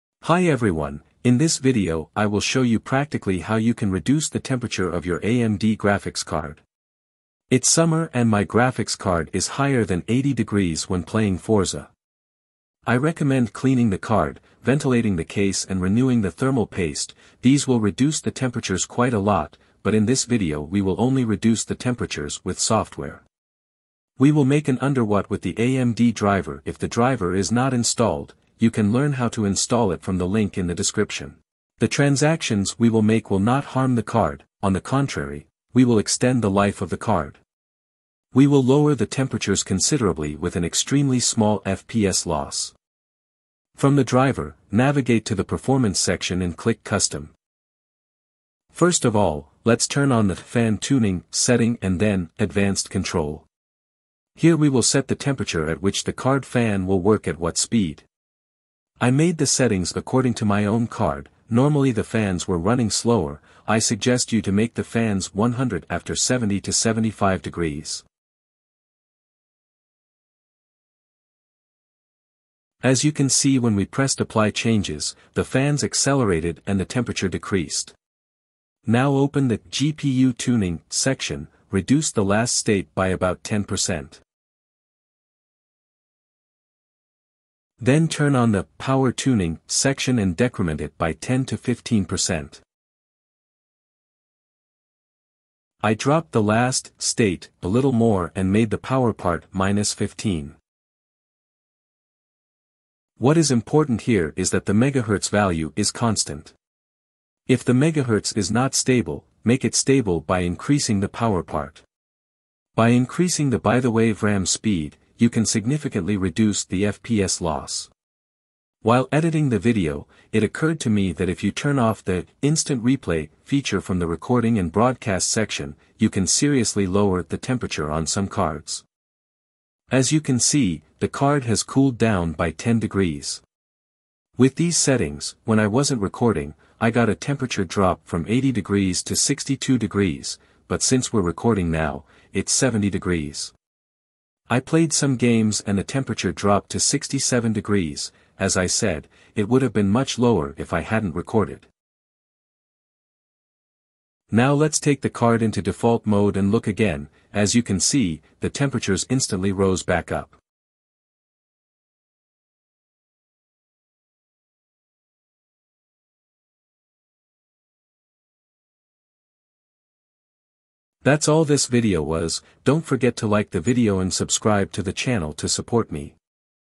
How to Lower Your AMD GPU Temperature
If you're struggling with high temperatures on your AMD GPU, you've come to the right place. In this video, we'll walk you through essential tips and tricks to help lower your GPU temperature, ensuring smoother performance and longer hardware life.

🔥 In This Video:

Optimizing Fan Speed: Step-by-step guide on how to adjust your GPU’s fan settings for better cooling.
Improving Case Airflow: Tips on how to enhance airflow within your PC case to reduce overall temperatures.
Applying Thermal Paste: Learn how to properly apply thermal paste to improve heat dissipation.
Cleaning Your GPU and Case: Essential maintenance tips to keep dust and debris from causing overheating.
Overclocking Considerations: What you need to know about overclocking and its impact on GPU temperature.
Whether you're a gamer, content creator, or just looking to get the most out of your AMD GPU, these practical steps will help you keep things cool under pressure.

If you have any questions or additional tips, drop them in the comments below—we'd love to hear from you!

Stay cool and game on! 🎮✨